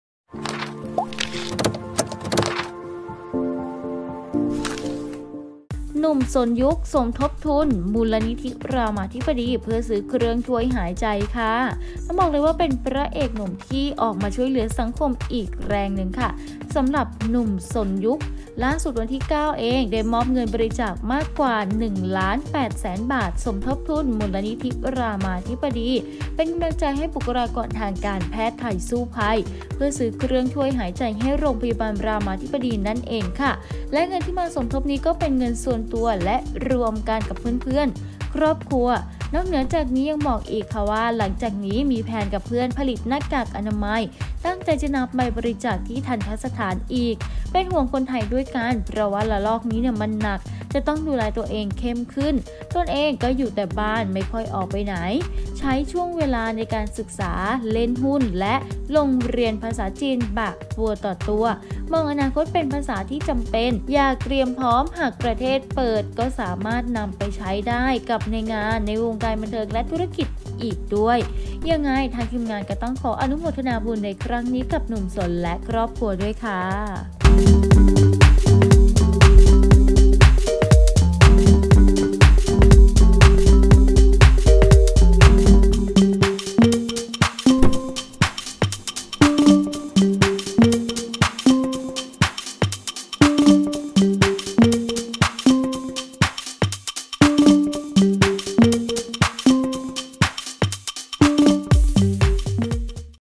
อนุโมทนาบุญ สน ยุกต์ สมทบทุนมูลนิธิรามาธิบดีฯ เพื่อซื้อเครื่องช่วยหายใจ!!
ข่าวดารา สน ยุกต์